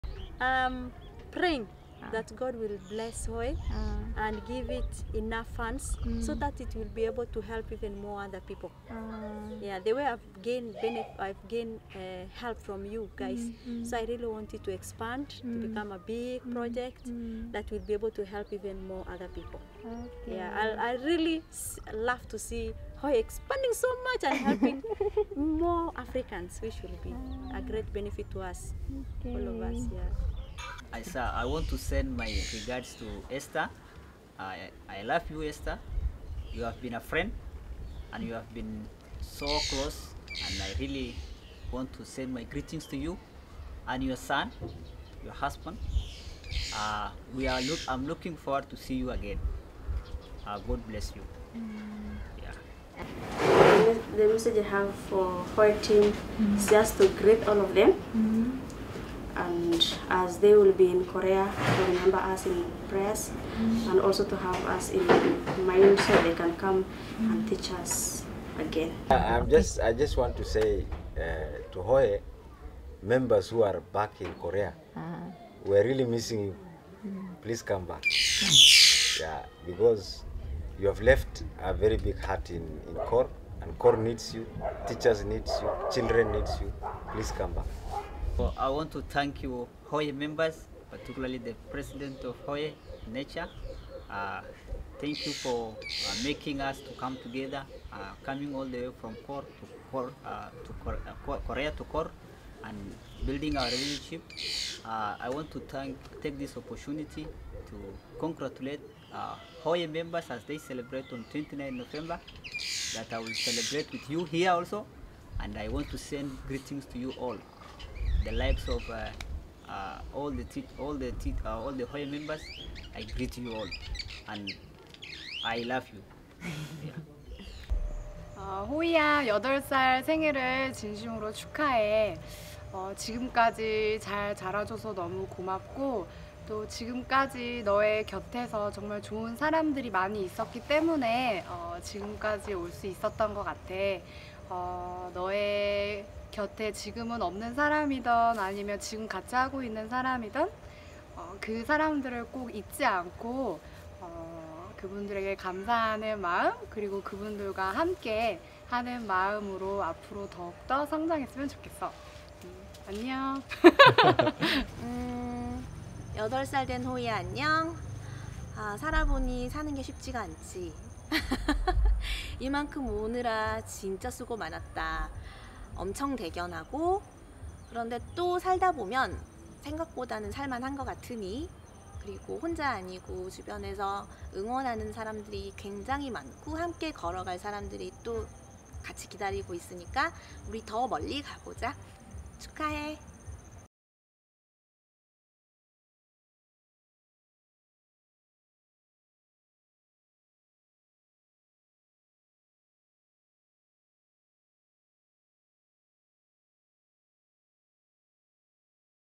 [HoE] 케냐와 우간다에서 온 생일축하영상
오늘로 8살을 맞은 호이에게 케냐 코어와 우간다 글루에 계신 선생님들이 축하영상을 보내주셨어요. 우리 함께 볼까요?